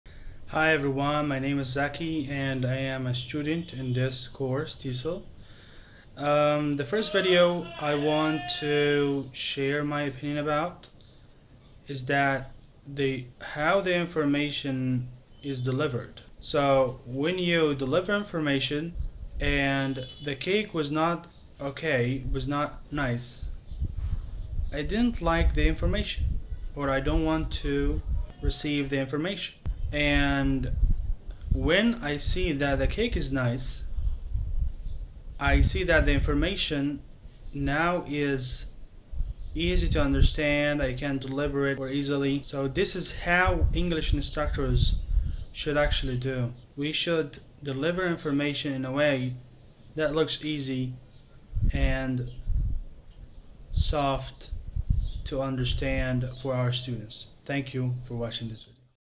TESOL first assignment “Language is like cake”
TESOL stands for Teaching English to Speakers of Other Languages
Think about the metaphor “Language is like cake”. I saw the first video. I noticed the following: delivering information must be a piece of cake, I mean easy to follow. When you deliver the information to your students, do not give it to them as a bad cake that no one wants to eat. Moreover, put it easily and in a way that all students want to eat.